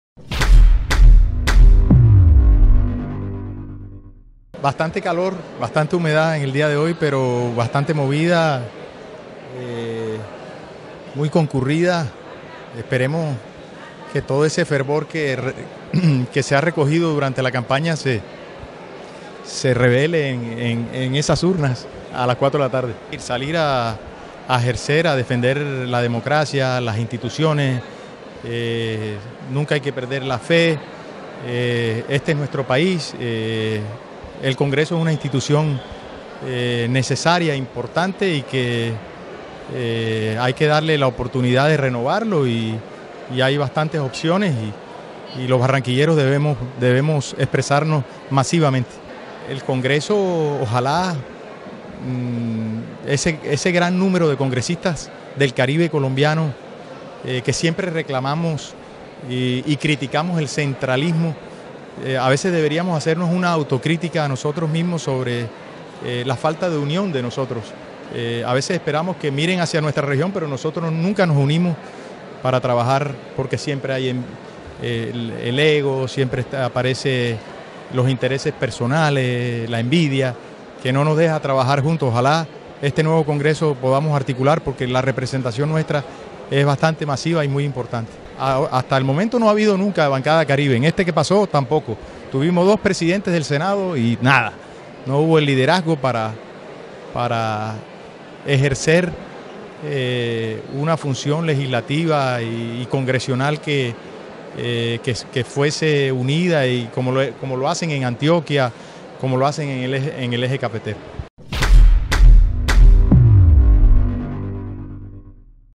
Arturo Char reconoce que bancada Caribe en el Senado no cumplió las expectativas.
Arturo Char, senador de Cambio Radical que busca repetir curul, acudió al puesto de votación del colegio Biffi en compañía de sus hijos, su esposa y su padre, el cacique electoral Fuad Char.

Luego de depositar su voto, atendió a los medios y reconoció que la bancada Caribe en el Congreso, cuyas principales figuras son los 31 senadores electos para que el cuatrienio que está a punto de terminar, termina con más pena que gloria.